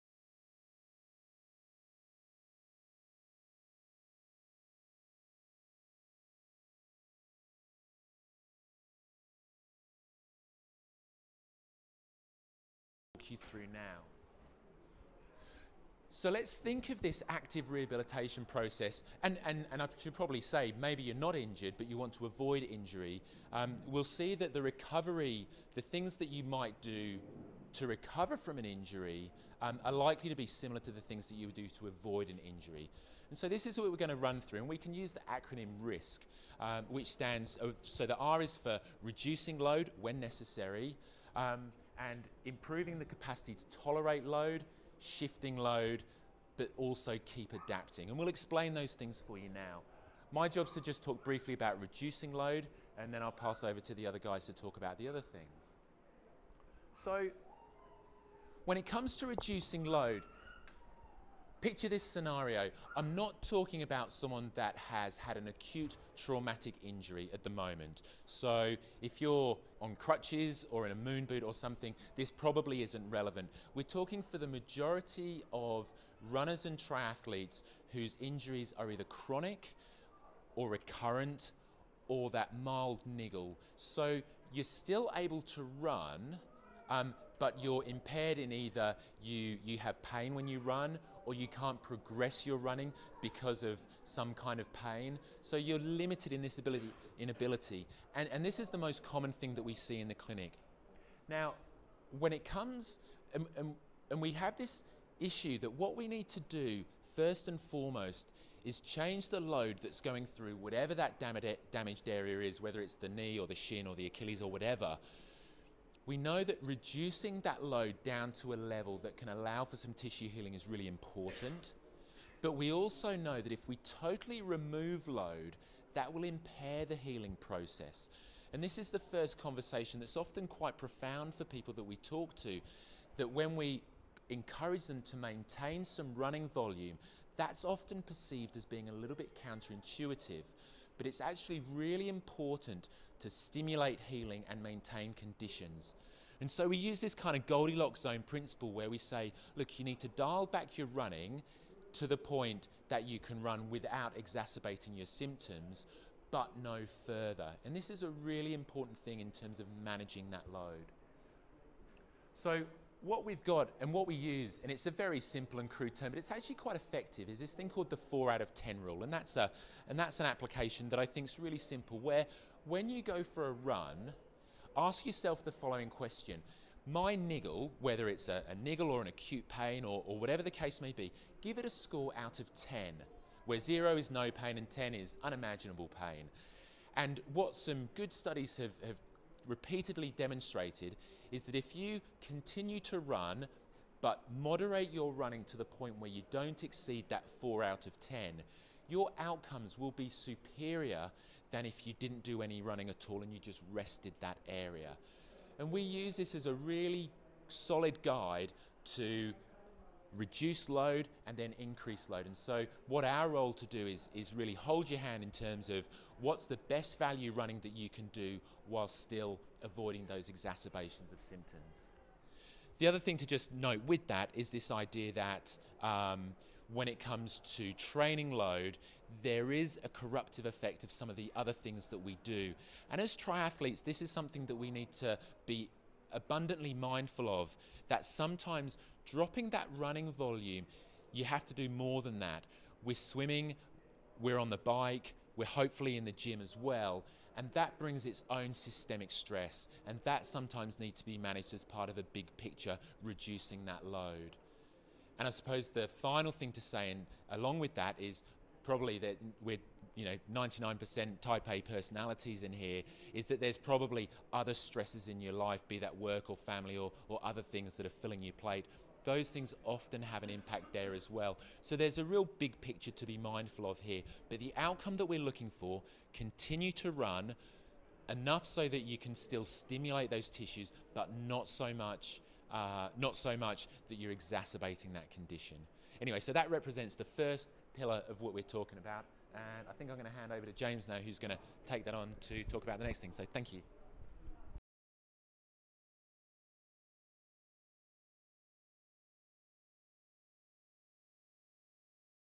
Dr James Debenham RISK reducing load 0
Join Star Physio's resident overuse and tendon injury expert Dr James Debenham (PhD) as he describes injury prevention and management in runners. Star Physio are proud to be the official physiotherapy and injury prevention partner for the Chevron City to Surt for Activ.